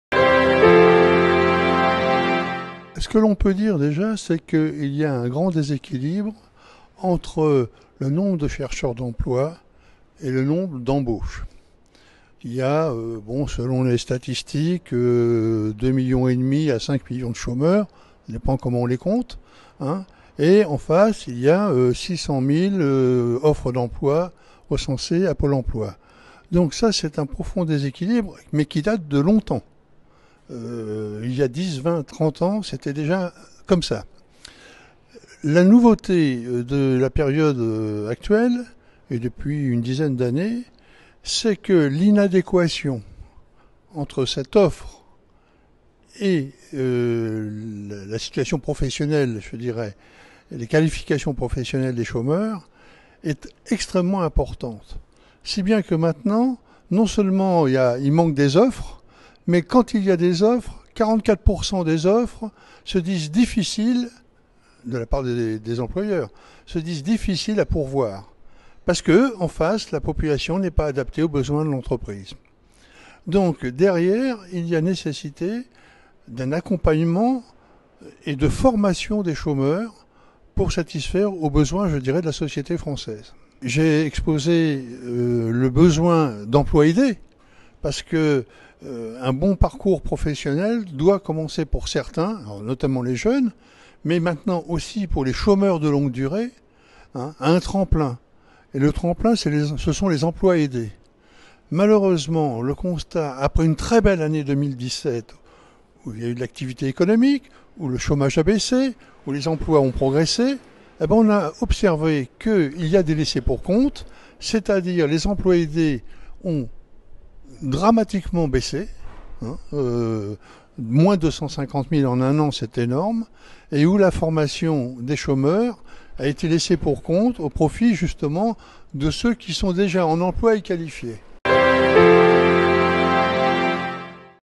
Jean-Pierre Revoil - Colloque SNC 2018
Les chiffres clés de l'emploi et données du chômage
par Jean-Pierre Revoil, membres du Pôle plaidoyer de SNC

Interview réalisée lors du colloque : « La santé des chercheurs d’emploi, enjeu de santé publique » jeudi 20 septembre 2018 à la Cité de refuge à Paris pour accompagner la sortie du second rapport sur l'emploi et le chômage et ses impacts de l'association Solidarités Nouvelles face au Chômage.